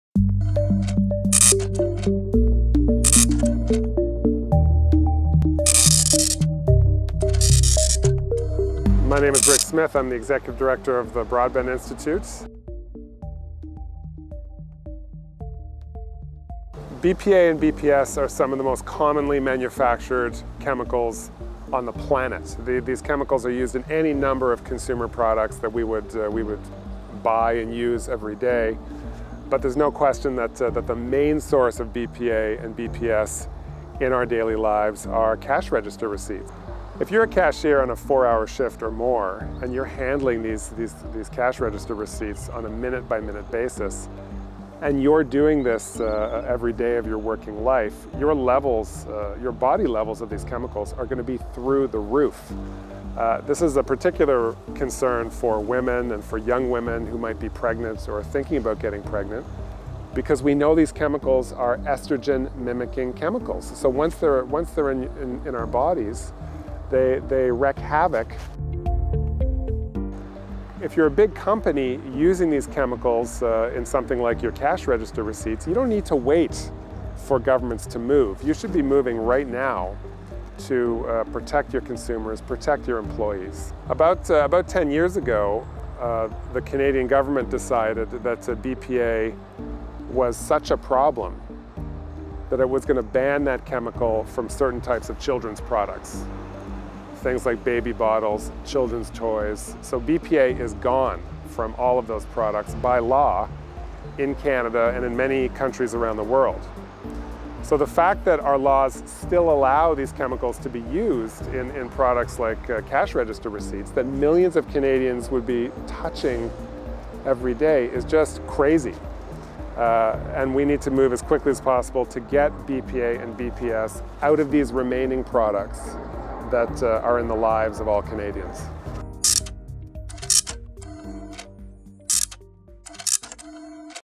Toxic Receipts: Health Effects
Unassuming toxic chemicals surround our everyday lives and are absorbed into our bodies. Some of these unassuming chemicals are bisphenol A (BPA) and bisphenol S (BPS). BPA/BPS are used in many applications, including on thermal paper cash register receipts. This is of great concern to retail workers, due to the high number of receipts that cashiers handle daily.

Rick Smith, Executive Director of the Broadbent Institute explains the dangers of these chemicals when absorbed into our blood stream.